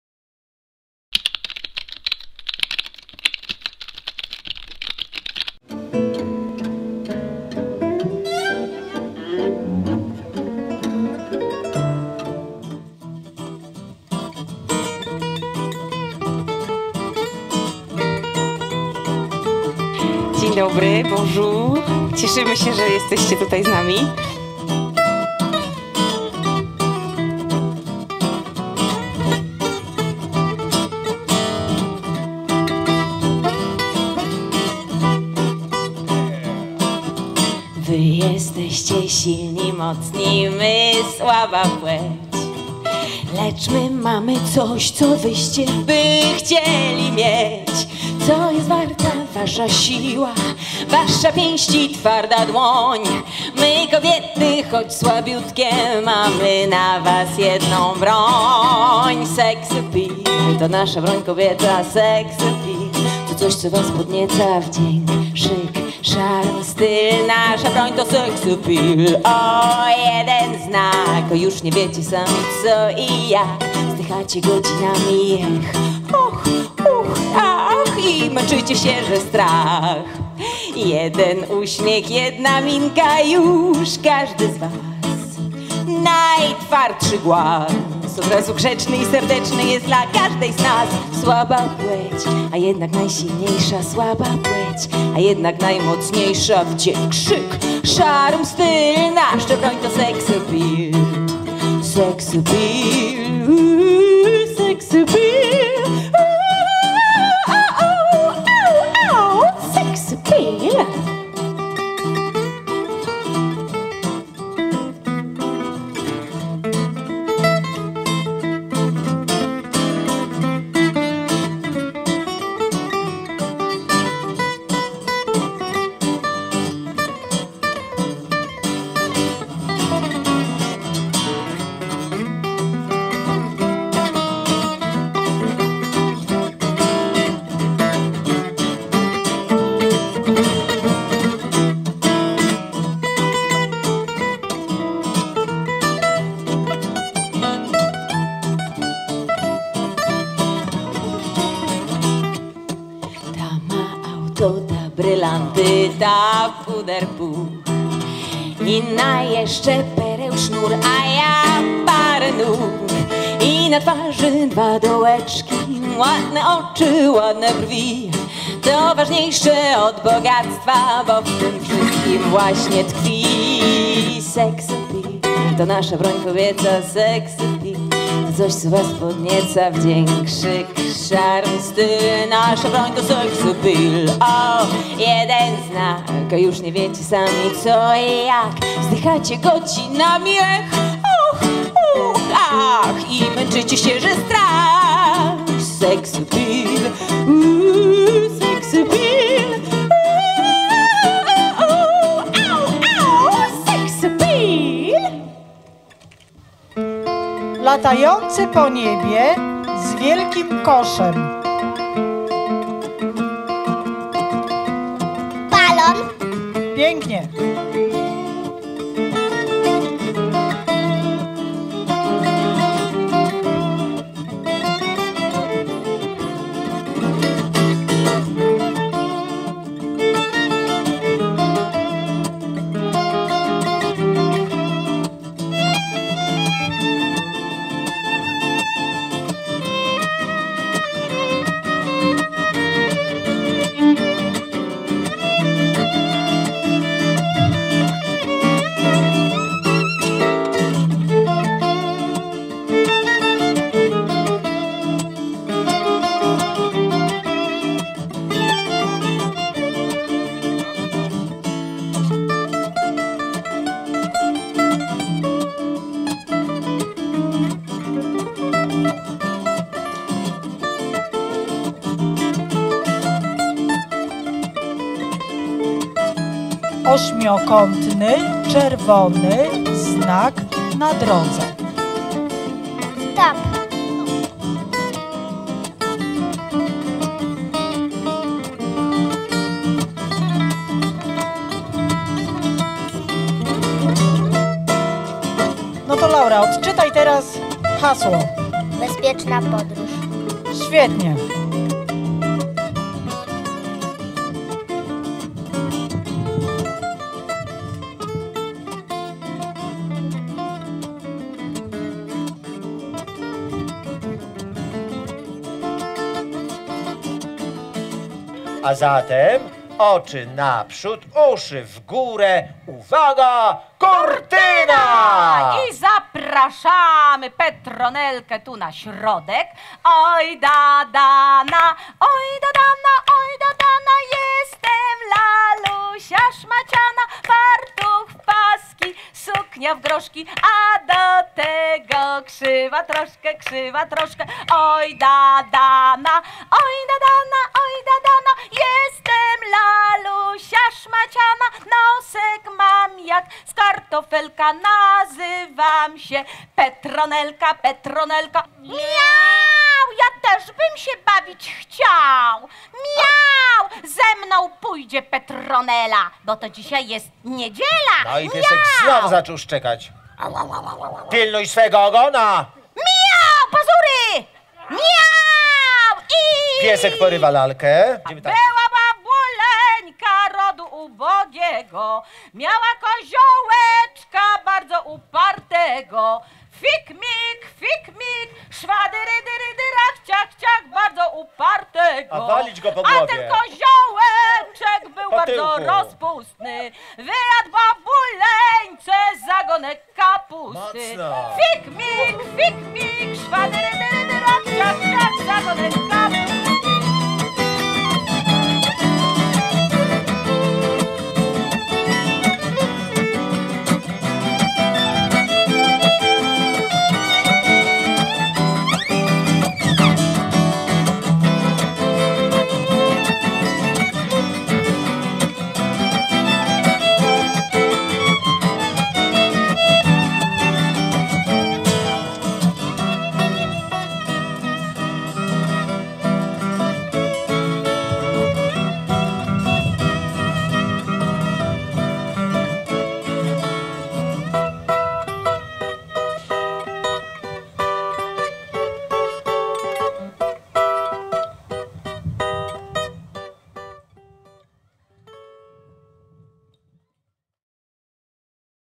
Sfera kultury Sianów 2017
Sfera Kultury odbyła się 13 sierpnia w Parku Miejskim im. Powstańców Warszawskich w Sianowie. Zobaczyliśmy niezwykły występ zespołu Twisted Fingers pod przewodnictwem Grzegorza Wypycha. Zespół ten to niespotykany tercet, którego muzyka oscyluje wokół jazzu cygańskiego, tzw. gypsy swing. Grzegorz Wypych to jeden z nielicznych w Polsce gitarzysta, wykonujący taką muzykę. Pozostali członkowie tria to wokalistka i skrzypaczka Lucy oraz gitarzysta Tom. Na zakończenie dnia odbył się występ Teatru Władca Lalek ze Słupska w spektaklu „Cztery mile za piec”. Interaktywne przedstawienie zaangażowało dzieci, które miały wiele atrakcji, gier i zabaw oraz dmuchane zabawki.